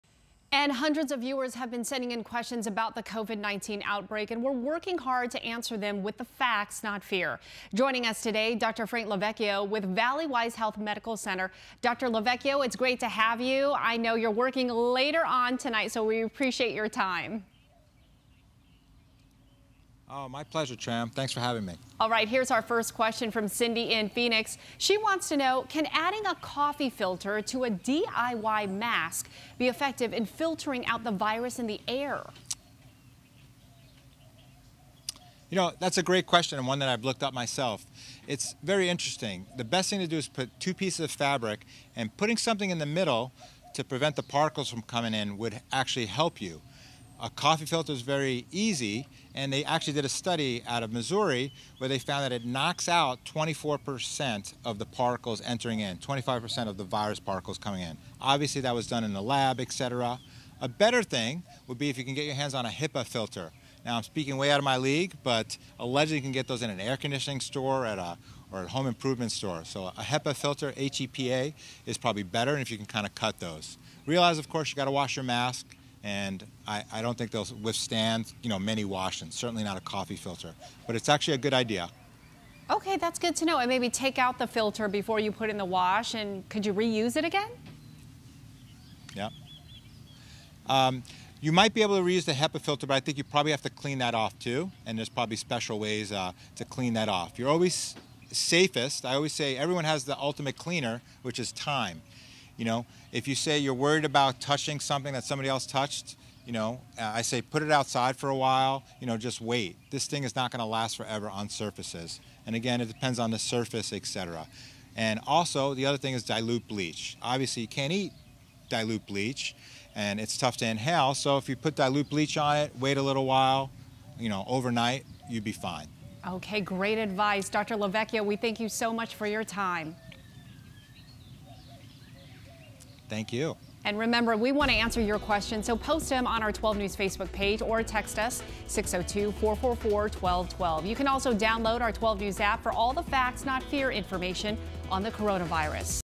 ASK THE EXPERTS: Can coffee filters help in DIY face masks?
Can adding coffee filters to your DIY face masks help improve their effectiveness? Tram Mai spoke with a medical professional to find out.